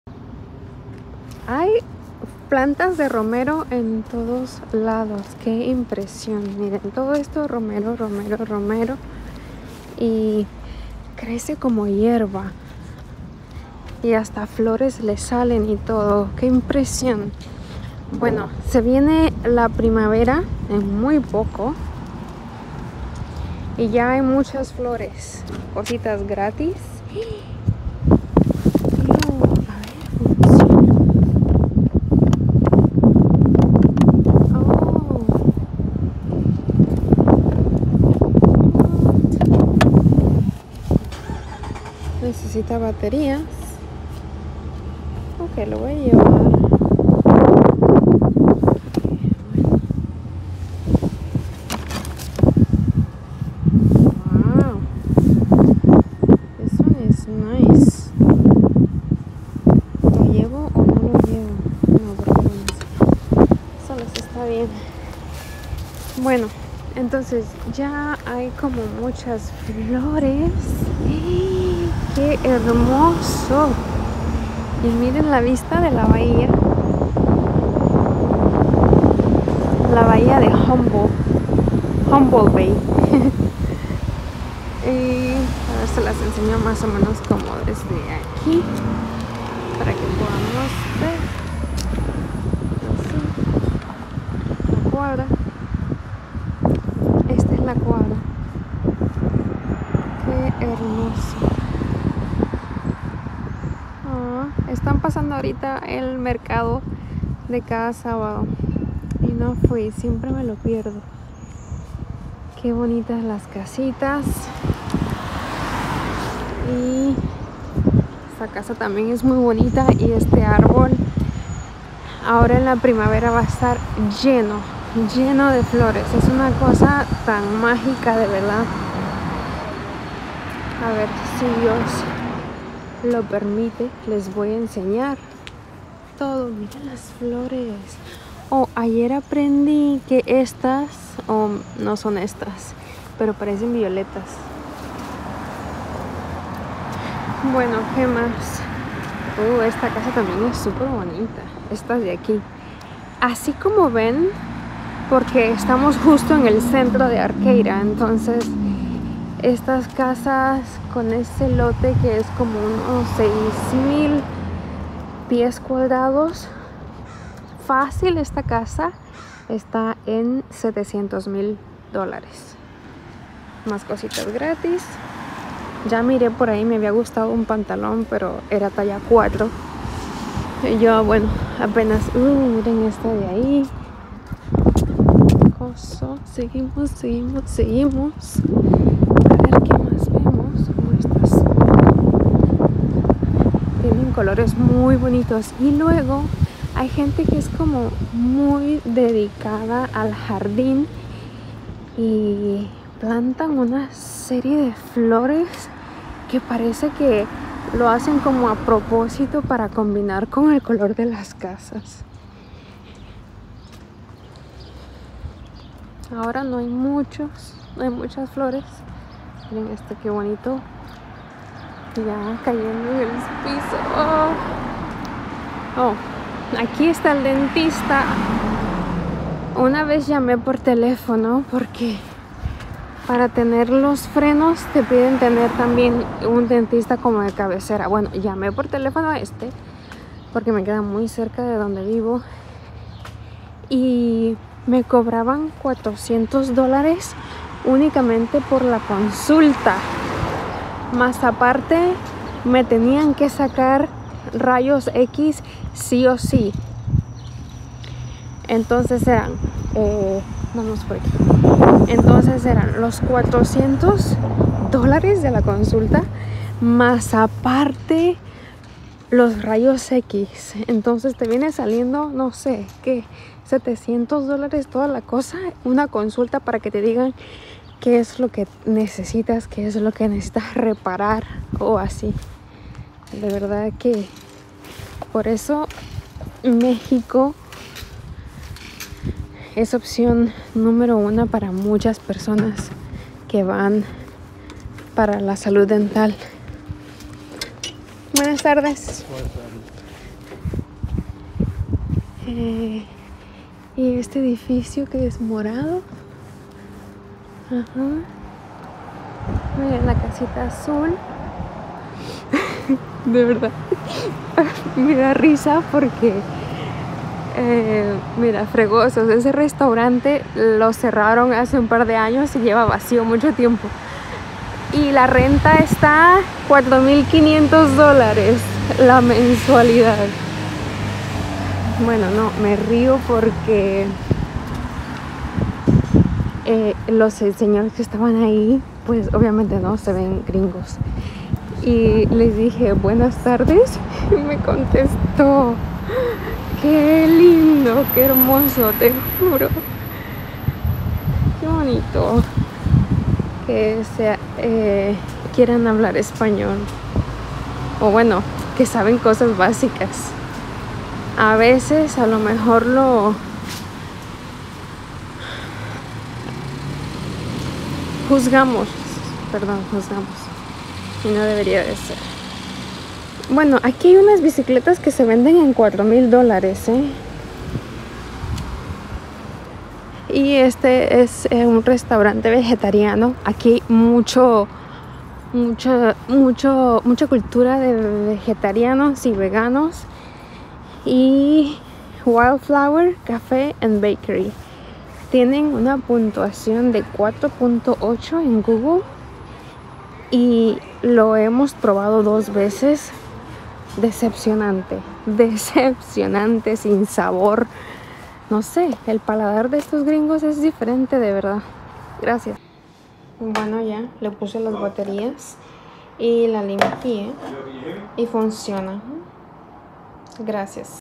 Caminando por las calles de Arcata California y encontrando cositas gratis
Arcata California es un pueblo tan magico, tiene unas vistas preciosas por todos lados.